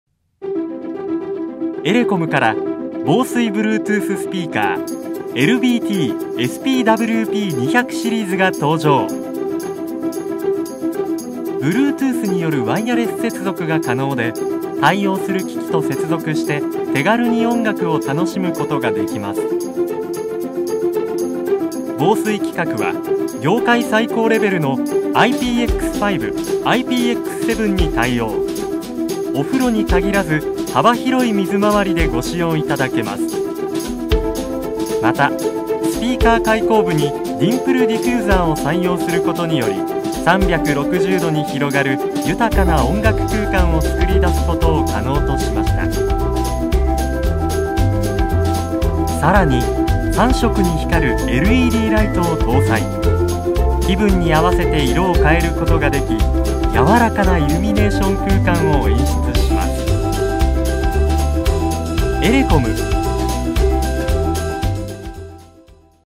LBT-SPWP200動画
エレコムのBluetooth防水スピーカー紹介動画です。